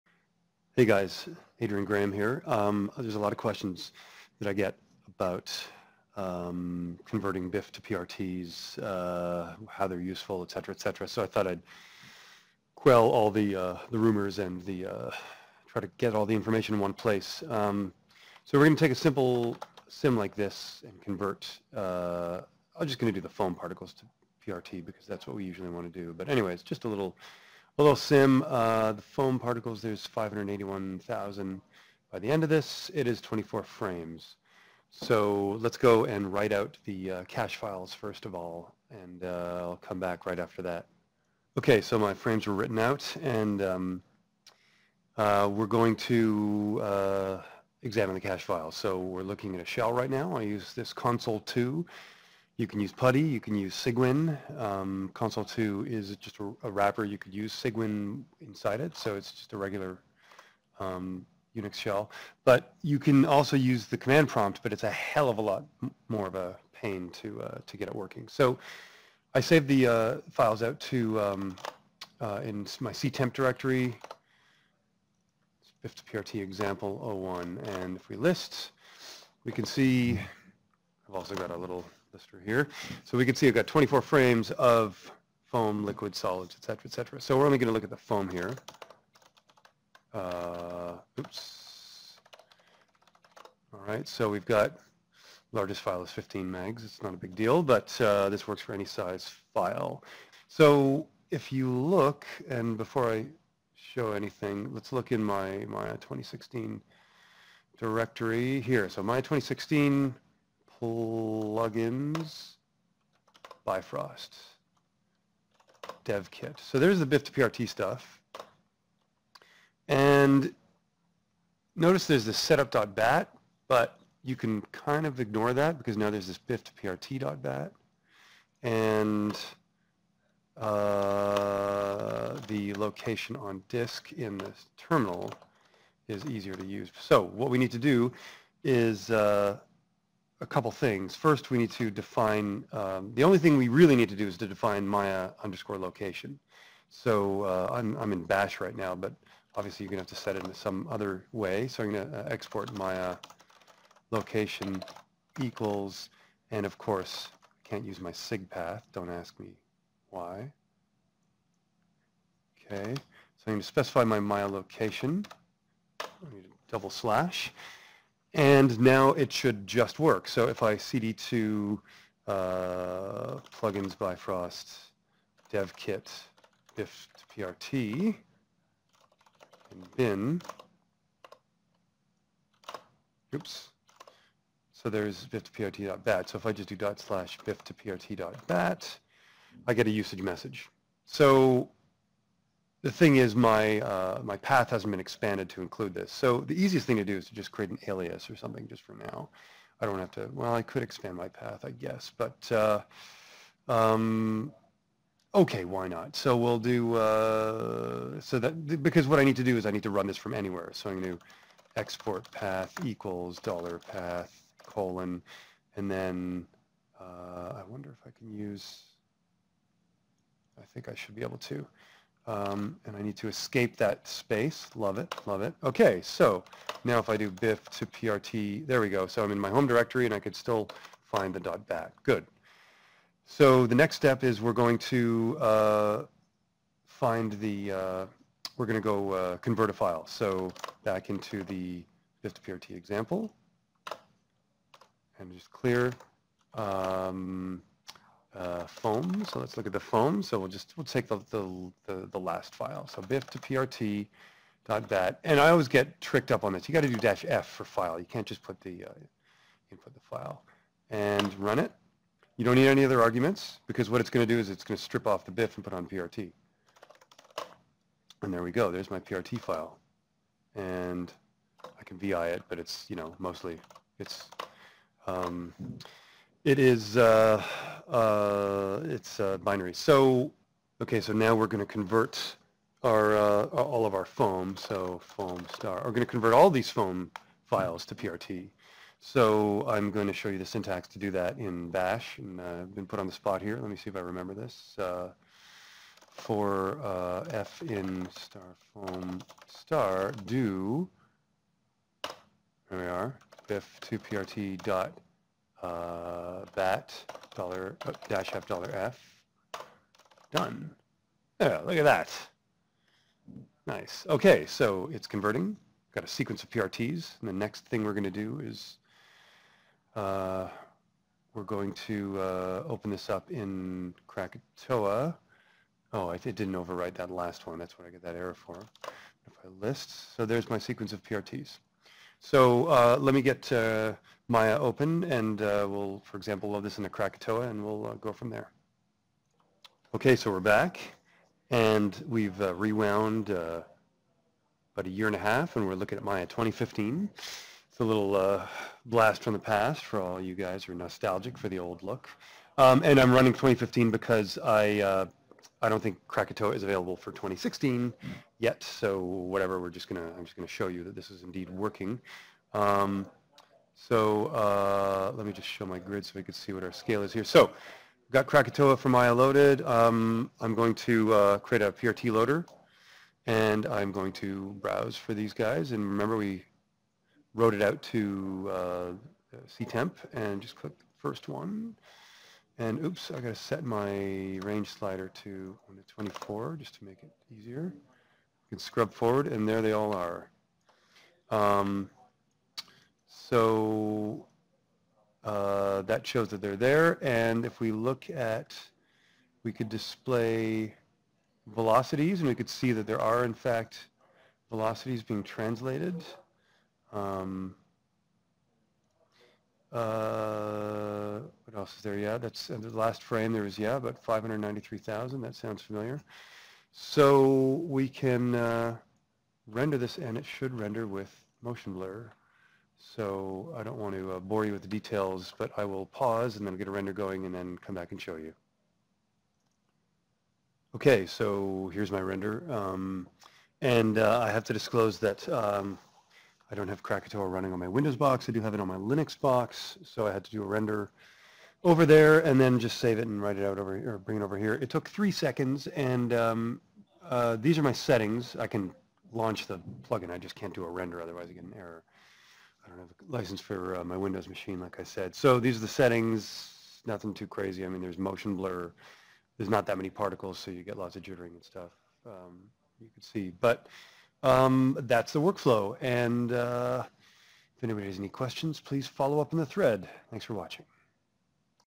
Maya 2016 Bifrost bif2prt workflow
In this video, I explain the workflow to convert your .bif files to .prt. It's quite simple, but there's a few things to help you set up your environment that will make your life much easier.